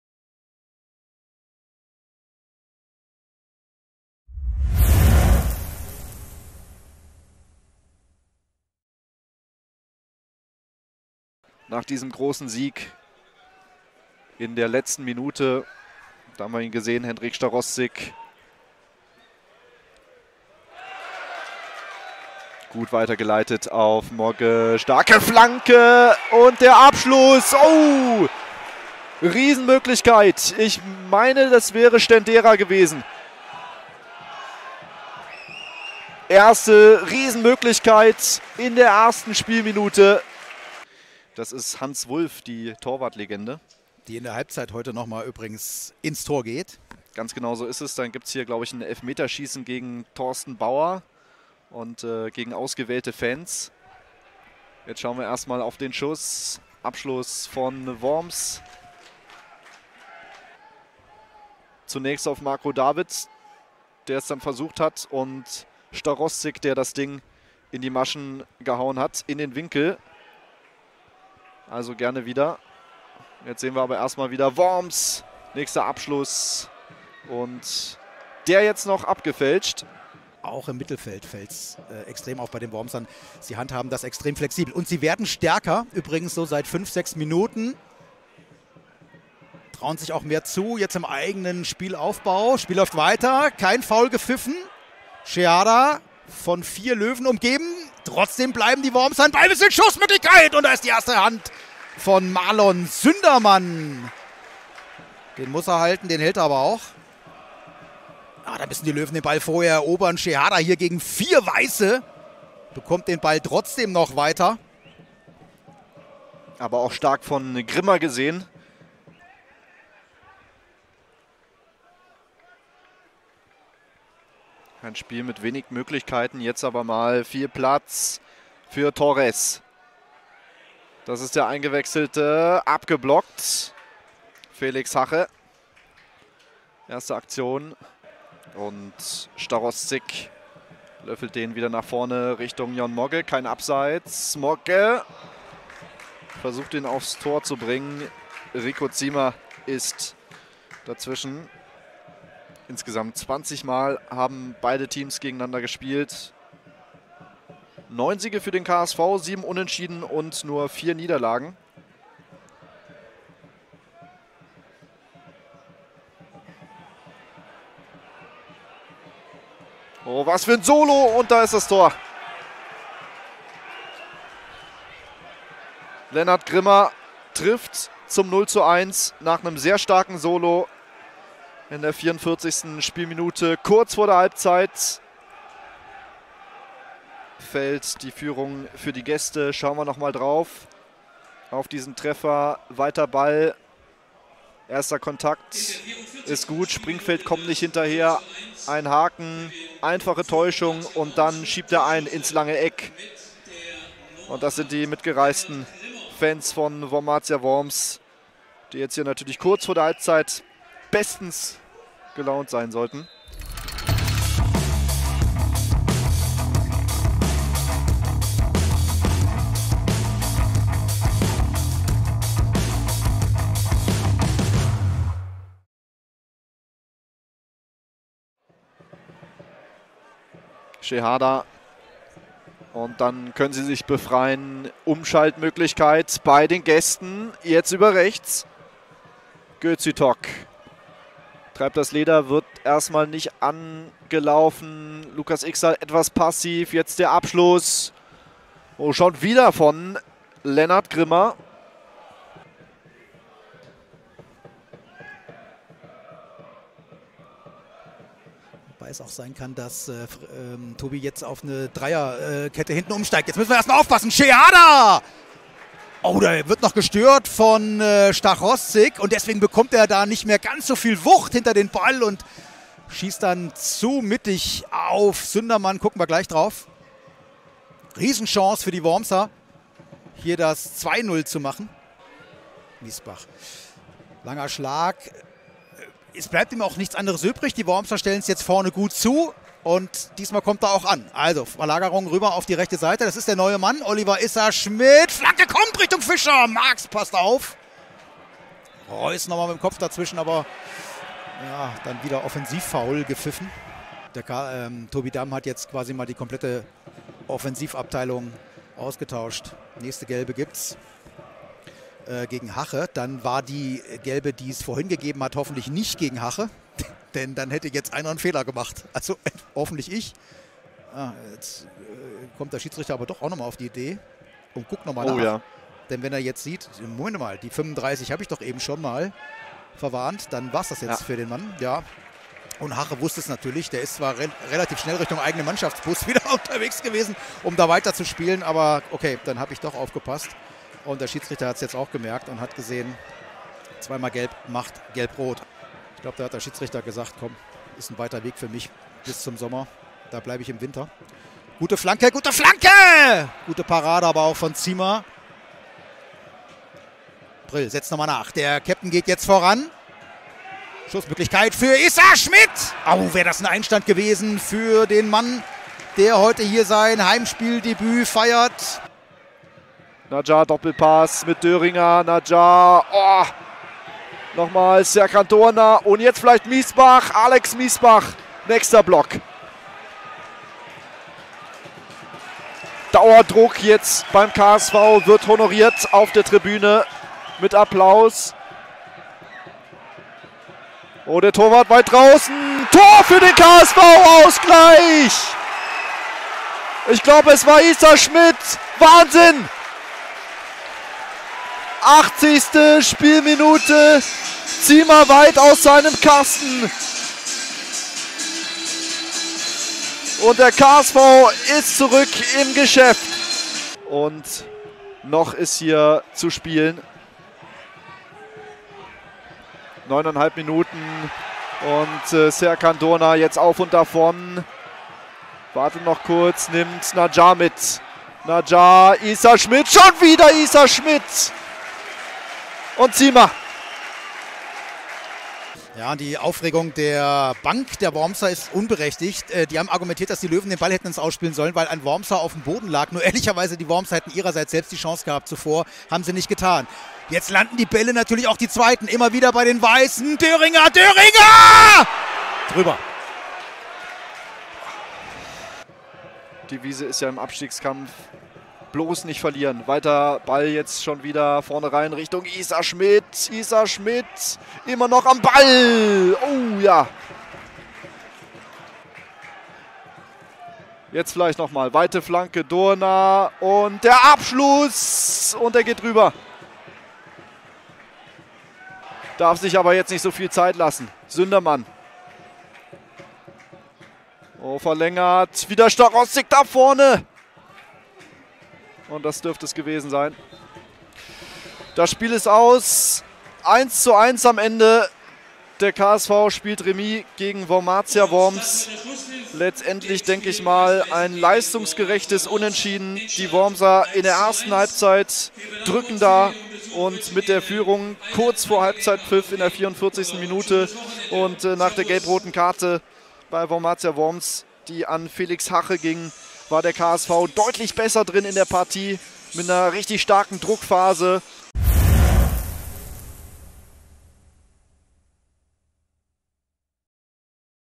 Hessen Kassel - Wormatia Worms 1:1 (19.11.2022)
Regionalliga Südwest | Saison 2022/23 | 17. Spieltag | 2.007 Zuschauer

0:1 Lennart Grimmer (44.)
1:1 Oliver Issa Schmitt (80.)

Aufzeichnung: Sporttotal / LEAGUES Livestream | Schnitt: WormatiaTV / Christian Bub